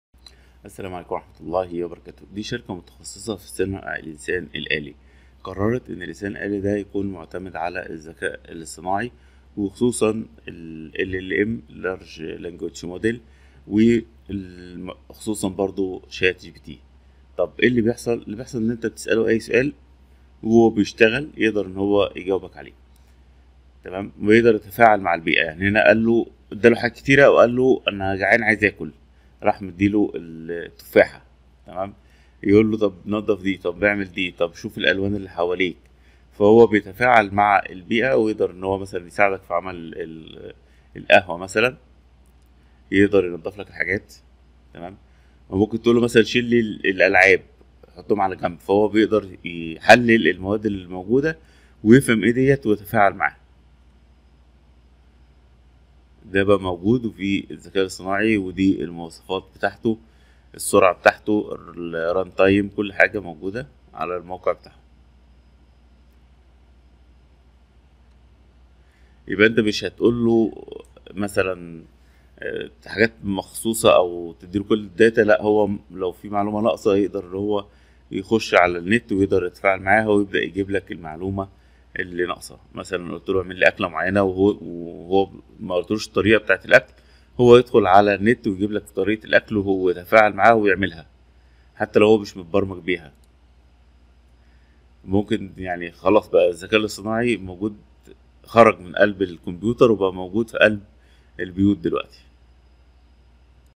تخيل معي تقنيات OpenAI مع روبوت متقدم في الوظائف الميكانيكية شن ممكن يديروا مع بعض، روبوت تفاعلي
❤️ روابط التواصل
✅ انستجرام